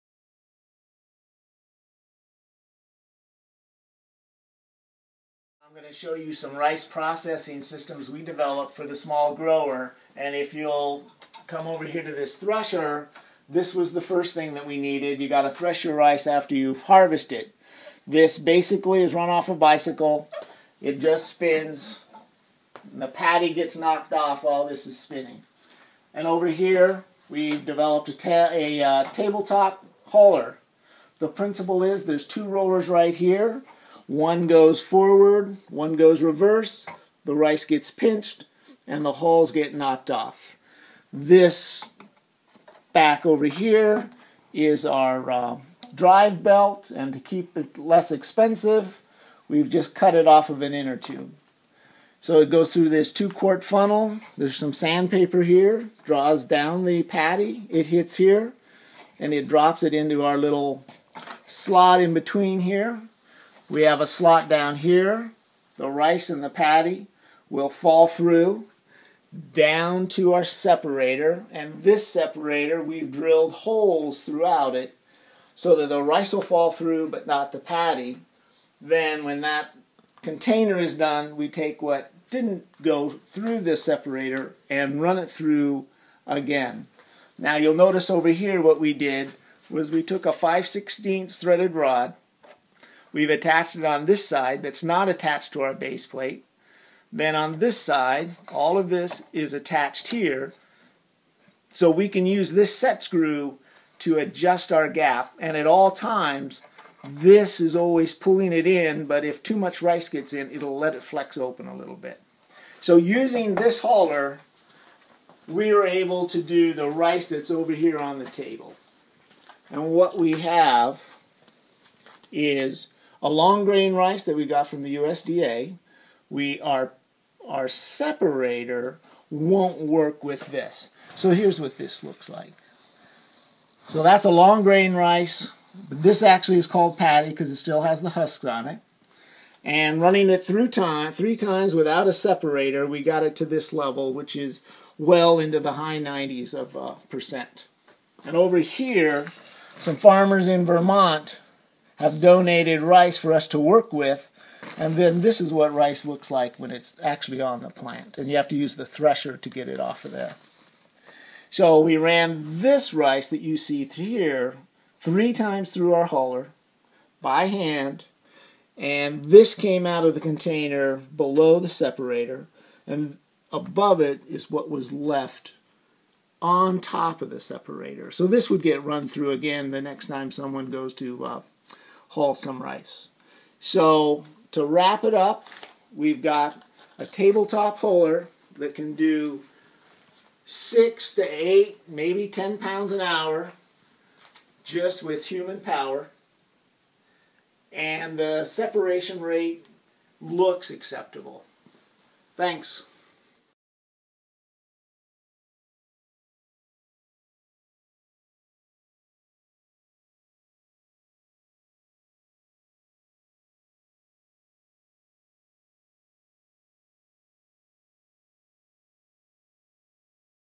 Small Scale Rice Husker
This is a general descripton of a human powered rice husker or rice huller.  Rice can be hulled (husked) by hand or it can be attached to a bicycle.  It hulls about 4 to 6 pounds per hour by hand and 10-15 pounds per hour with a bike with 70 to 80% of the rice hulled.